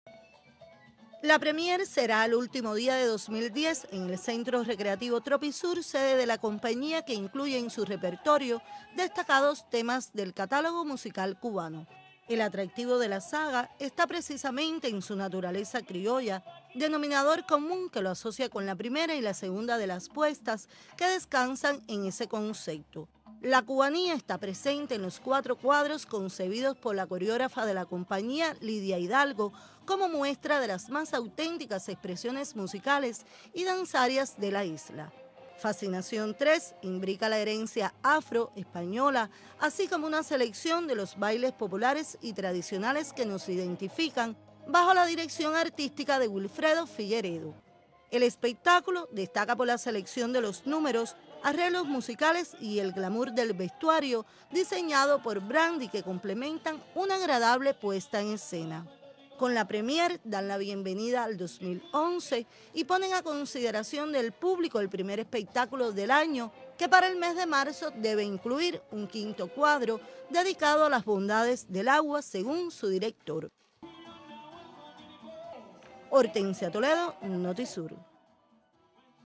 "Fascinación 3" en premier para finales de año en Cienfuegos
Wilfredo Figueredo y su Compañía de Espectáculo Musicales estrenarán "Fascinación 3", la más reciente propuesta del colectivo, con la cual despiden y dan la bienvenida al próximo año en la ciudad de Cienfuegos.